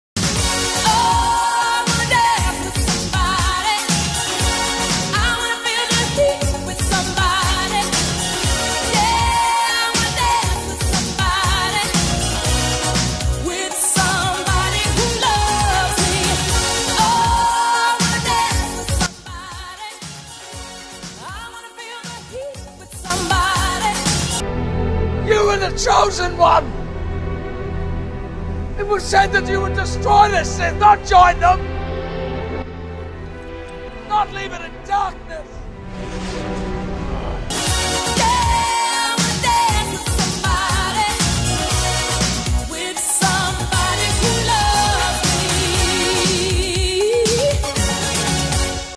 Marshall Appreciation Post
Just a quick little video to appreciate the greatest dancer our generation has to offer.